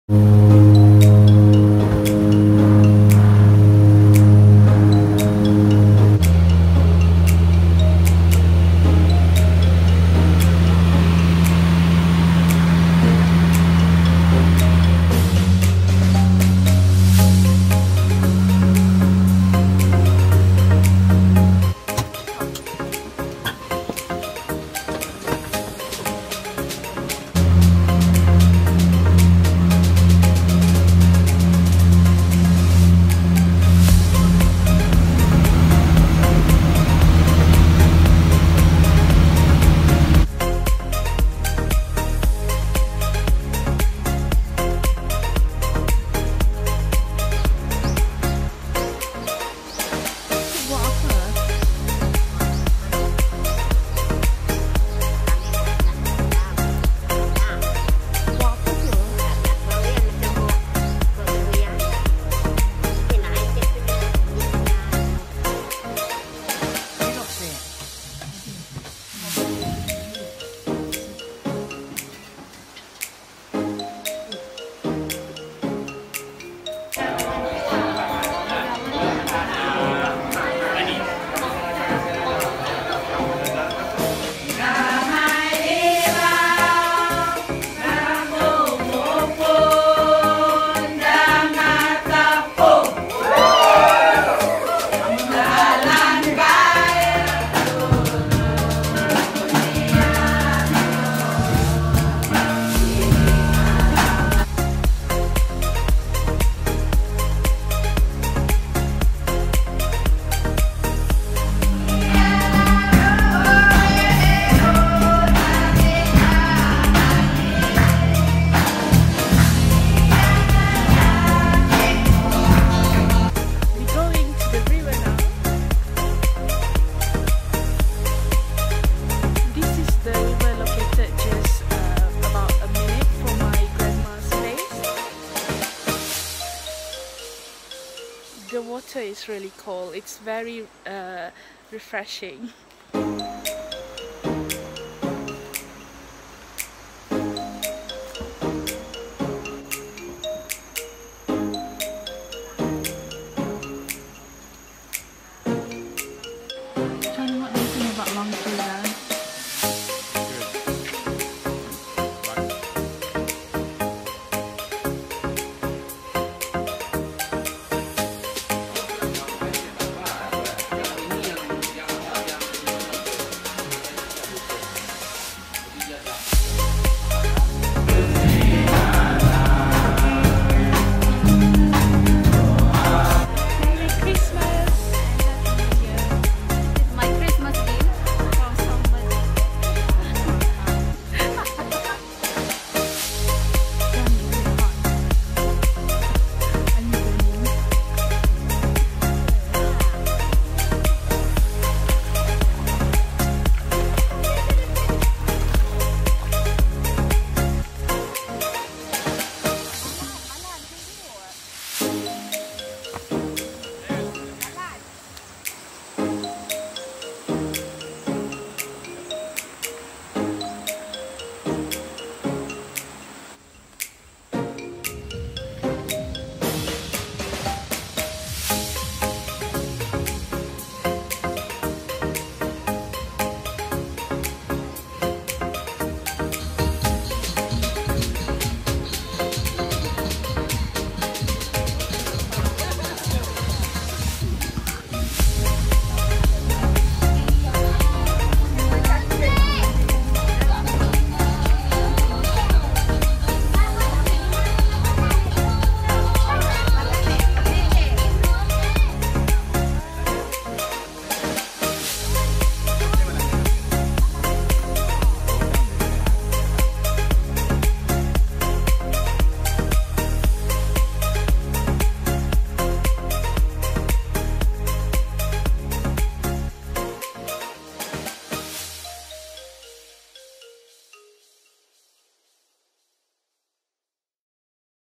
Long Seridan, Sarawak, Borneo Vlog (Christmas 2018)
This is our recent trip back to our hometown for Christmas. It's a lovely little village in the interior jungle of Sarawak called Long Seridan. Long Seridan is a Kelabit (one of the many ethnics in Sarawak) settlement in the Miri division of Sarawak, Malaysia.